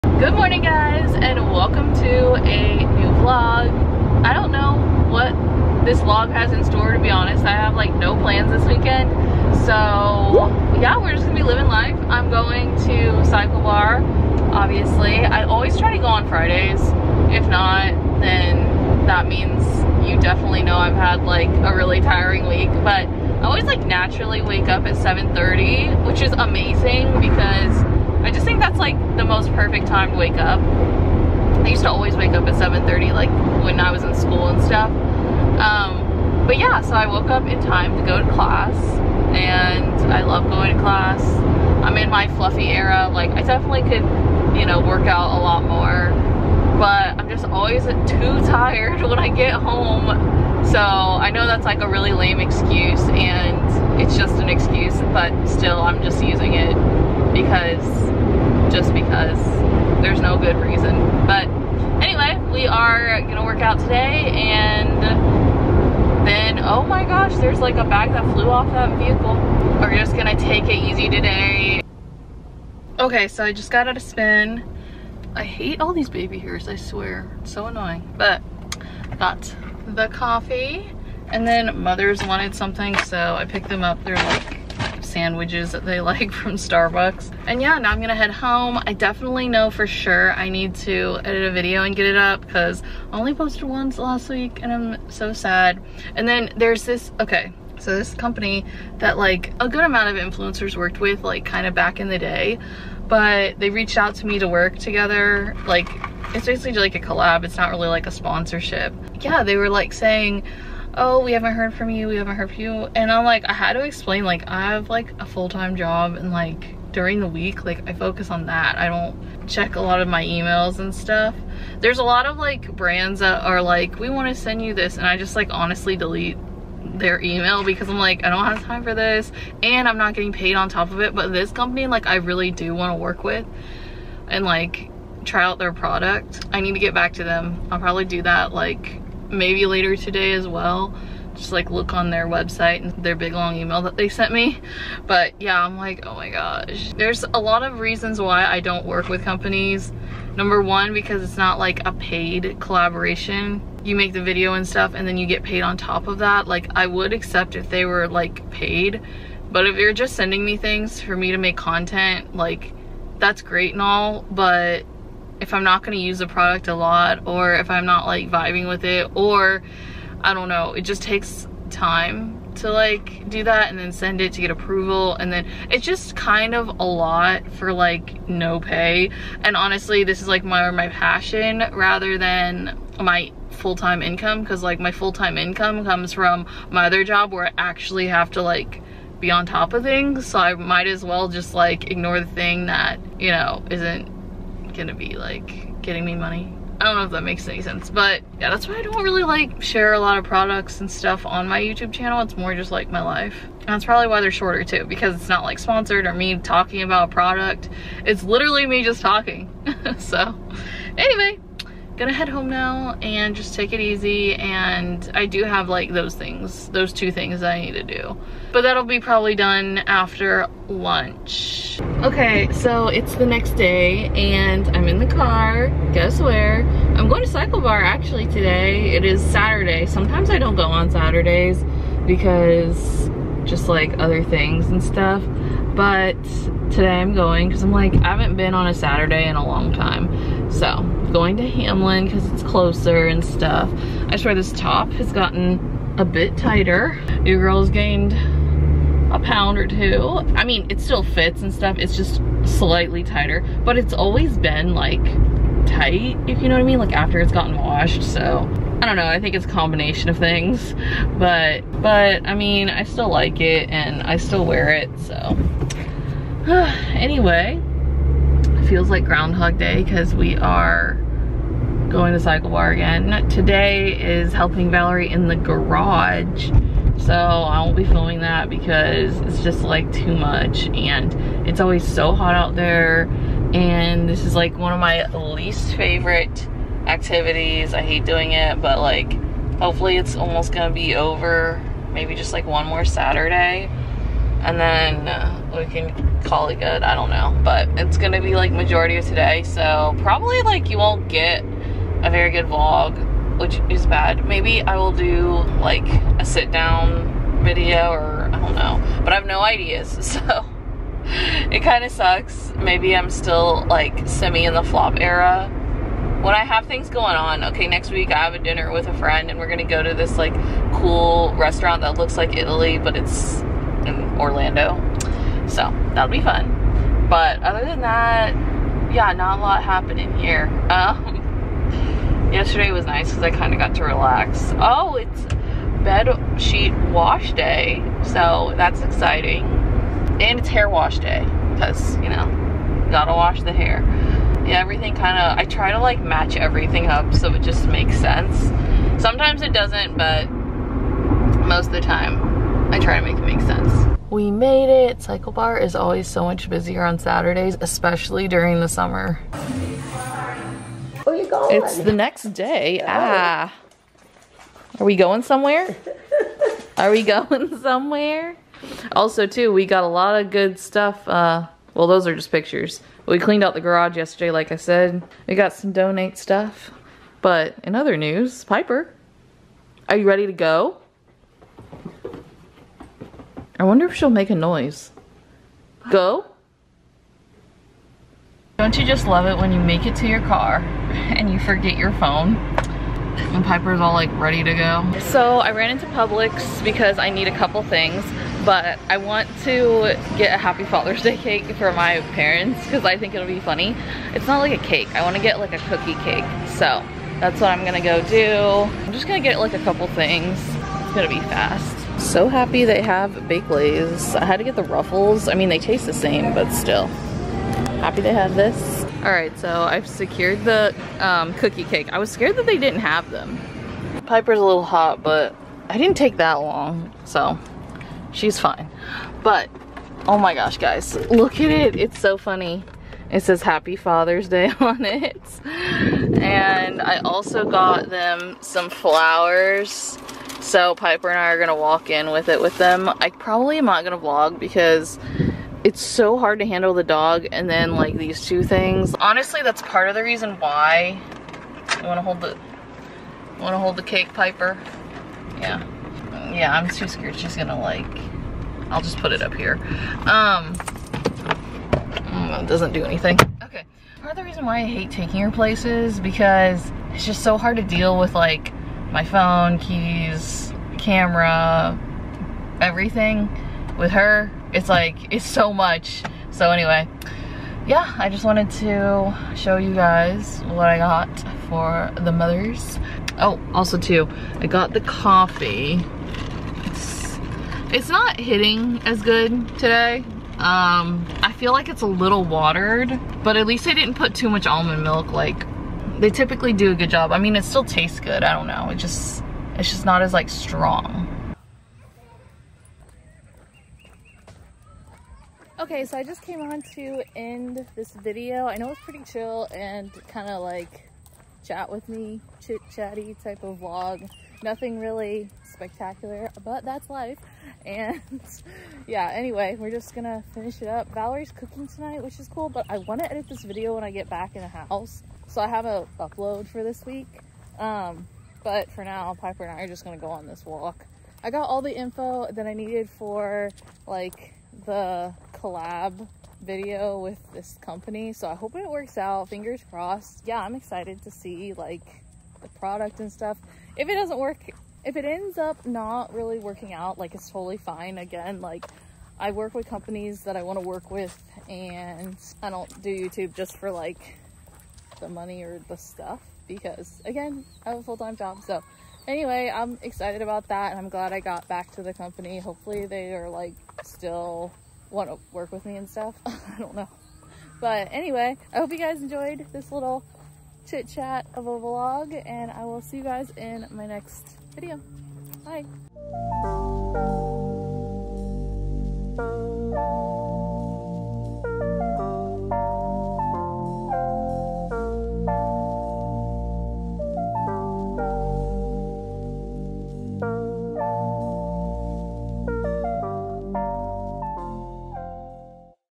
a chill weekend vlog
In this vlog I chit chat with you about creator collabs and life :)

❥  Alex